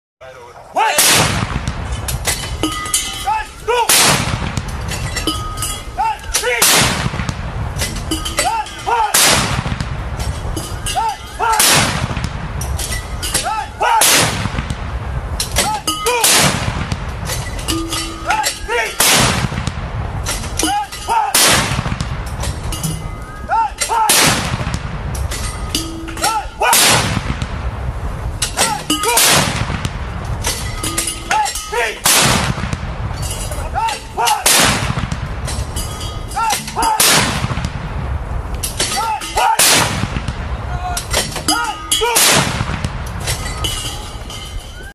Gunners of 223 Field Artillery Regiment executing the 21 Gun Salute on 75th Independence Day 2021.
The Gunners of 223 Field Regiment executed the traditional 21 Gun Salute on the occasion of glorious 75th Independence Day 2021.